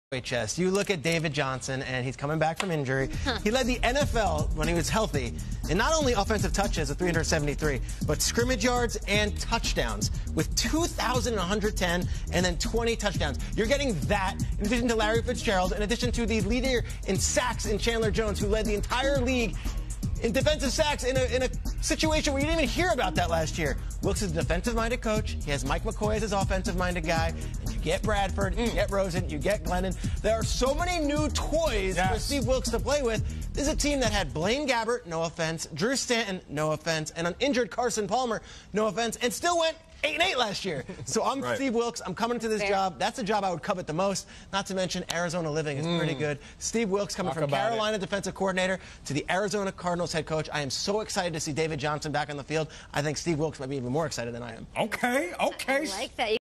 New head coach Cardinals is Steve Wilks | NFL News 2018
Channel about American football